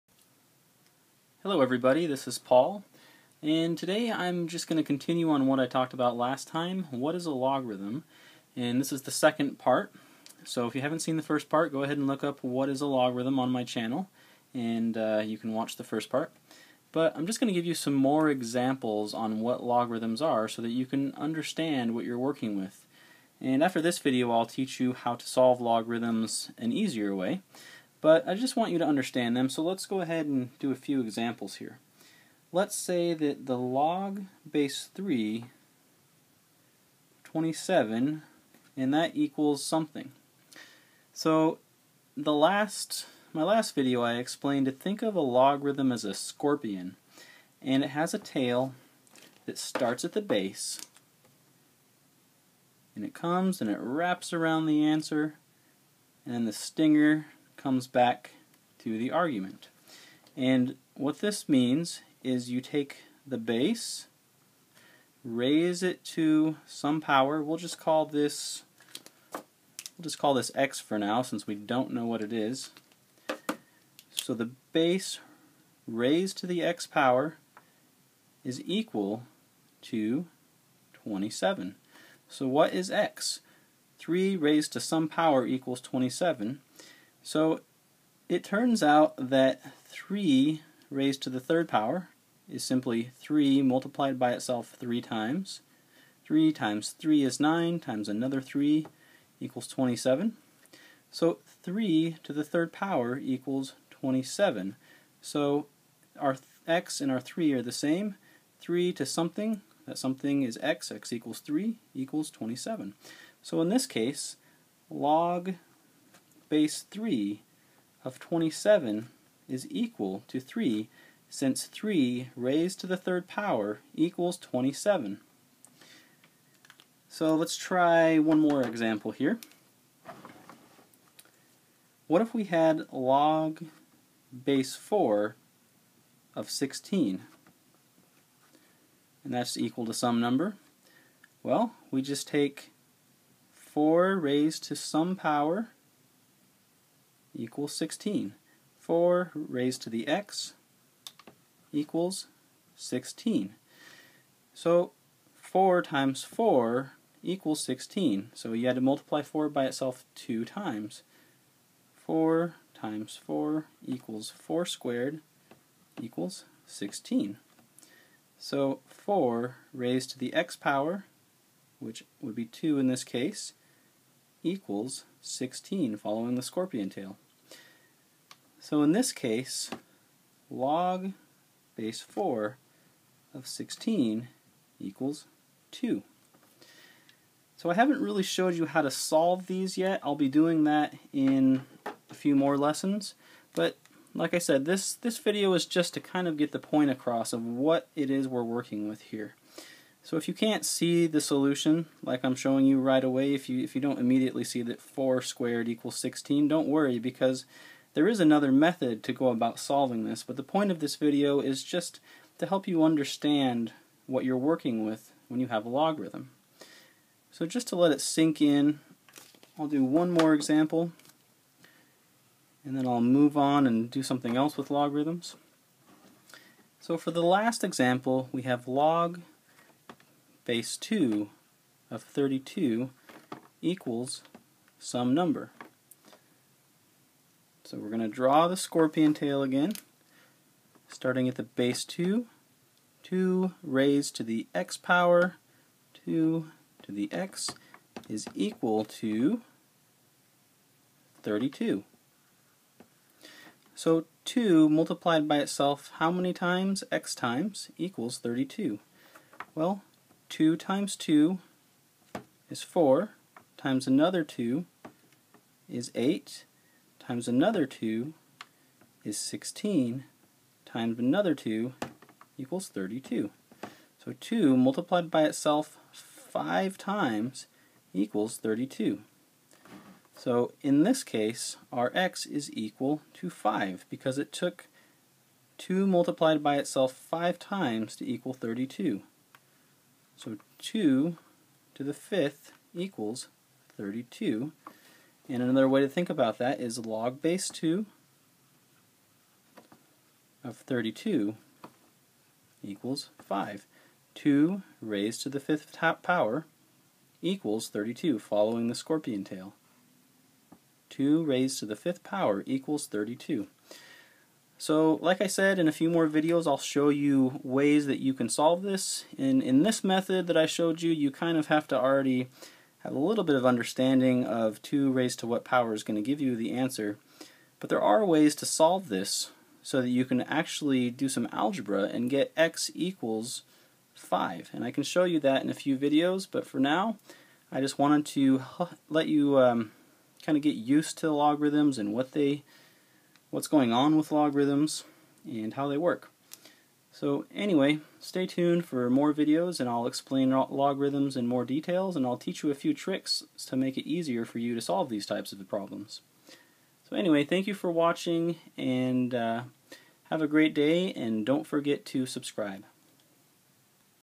What is a Logarithm ( Part 2 with More Examples ) : Logarithms, Lesson 2
This tutorial demonstrates 3 examples which help the viewer understand what a logarithm is.